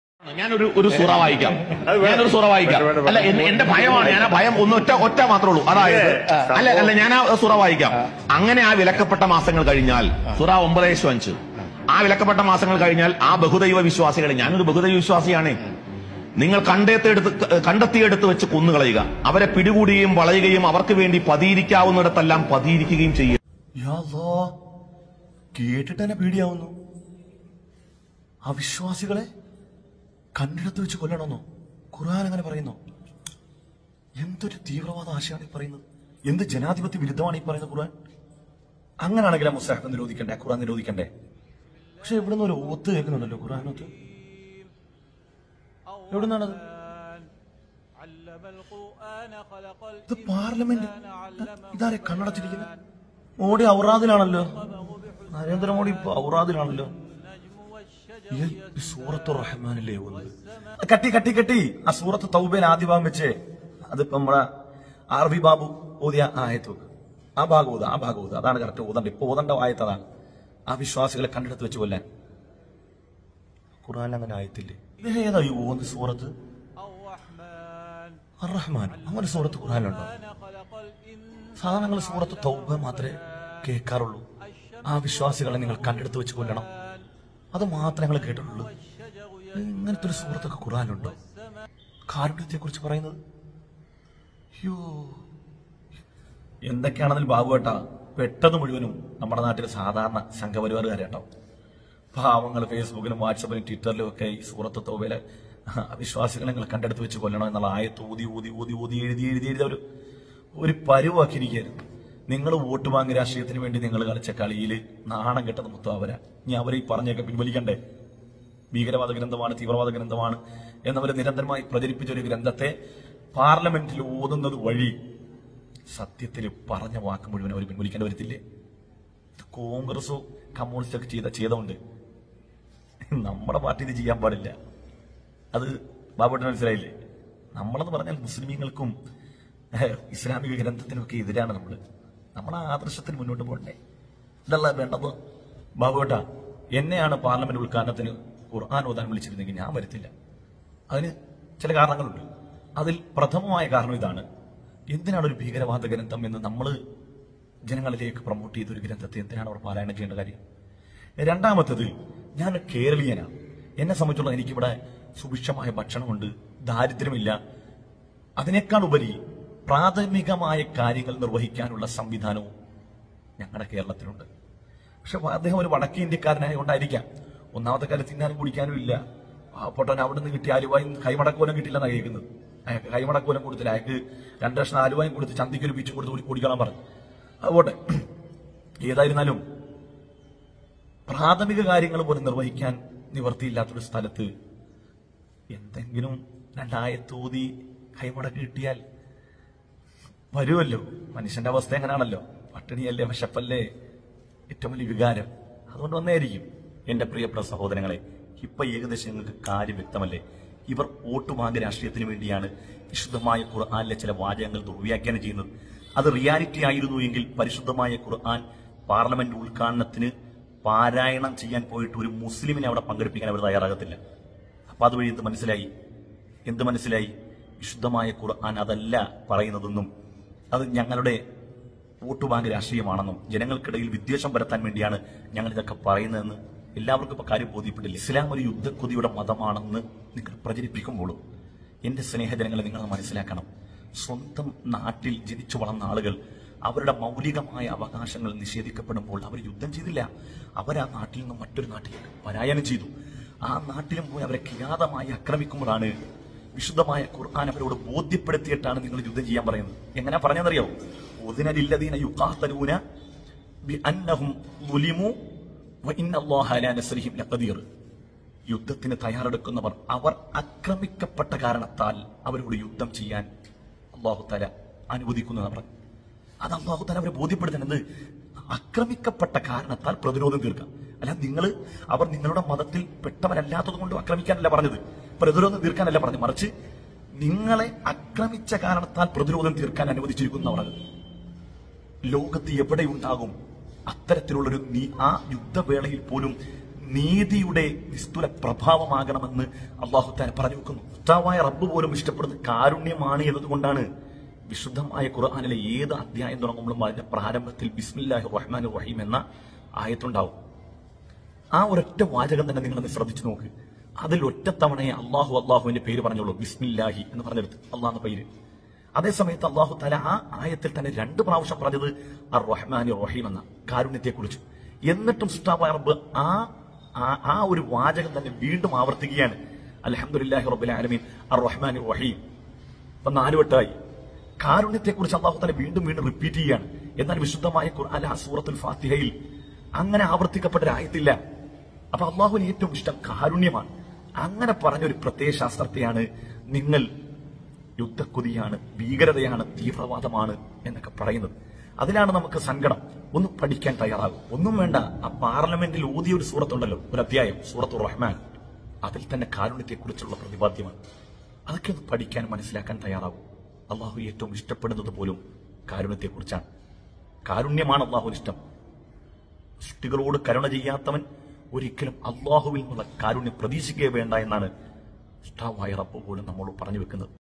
പാർലമെന്റ് ഉദ്ഘാടനത്തിലെ ഖുർആൻ പാരായണം !ANSARI ZUHRI ALAPPUZHA
പാർലമെന്റ് ഉദ്ഘാടനത്തിലെ ഖുർആൻ പാരായണം ! എന്തിനാണ് അവിശ്വാസികളെ കണ്ടിടത്ത് വെച്ച് ആക്രമിക്കാൻ പറഞ്ഞൊരു ഗ്രന്ഥം അവിടെ പാരായണം ചെയ്യുന്നത് " ?